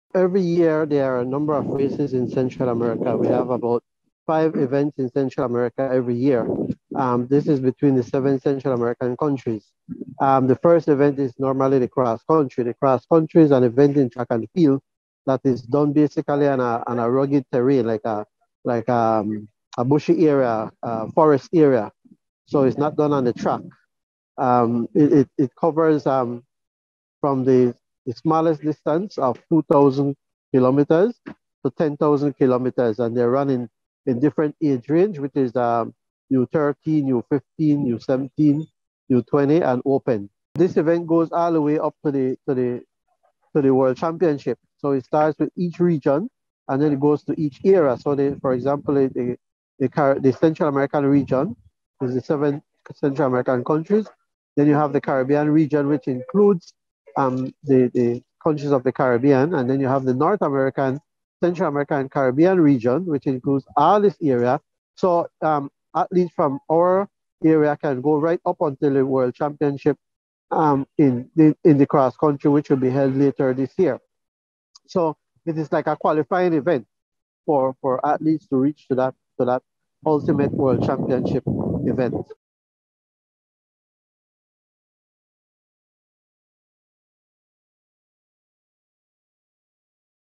Belizean Athletes Travel to El Salvador for the Central American Cross Country Championship | PT 1
Yesterday, thirteen Belizean athletes left for El Salvador to participate in the Central American Cross Country Championship that will be held over the weekend. The two-day event will see athletes from the seven different countries in Central America vie for titles in five different categories. The winners of the track and field events will then be moving on to the world championship event that will be held later in the year. Deon Sutherland, President of the Belize Athletics Association says the athletes are looking forward for capture medals for the country.